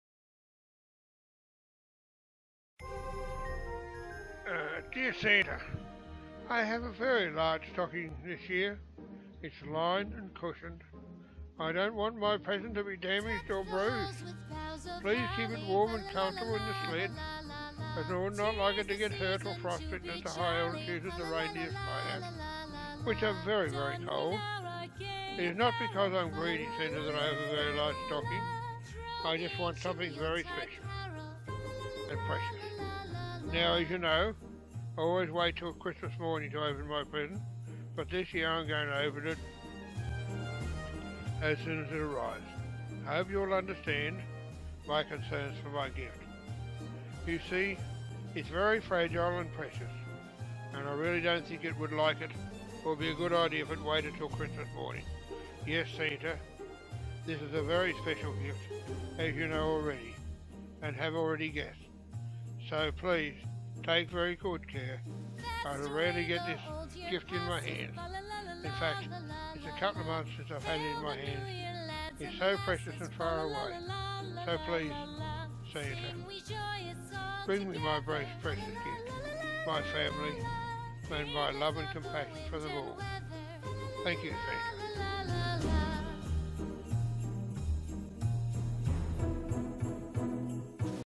My letter to Santa 2018
we lost one our children this year he slipped though our net of protection .Jason  was 19 years old only a month off his 2o th  birthday .the combination of a medical drug he was put on and his desire to help other cost him  his life .